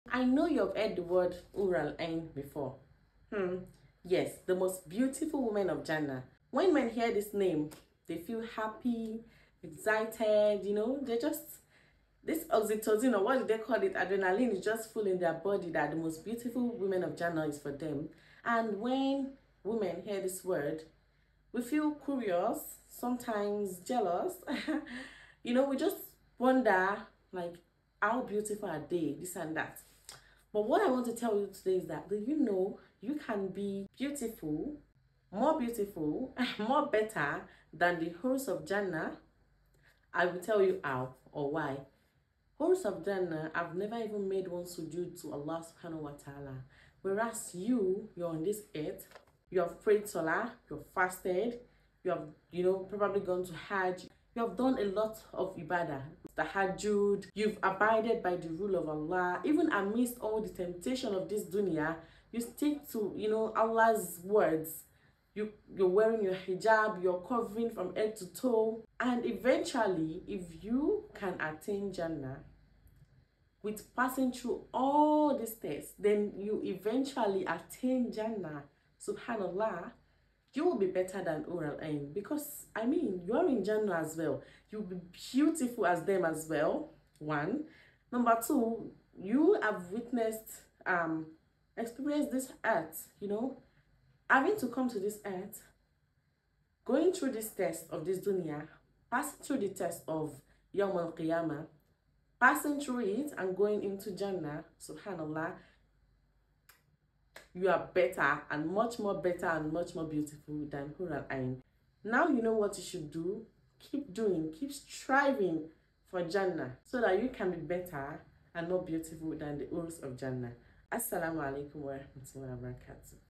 Hoors of jannah 🥰
The most beautiful women of Jannah are called Hoor Al Ayn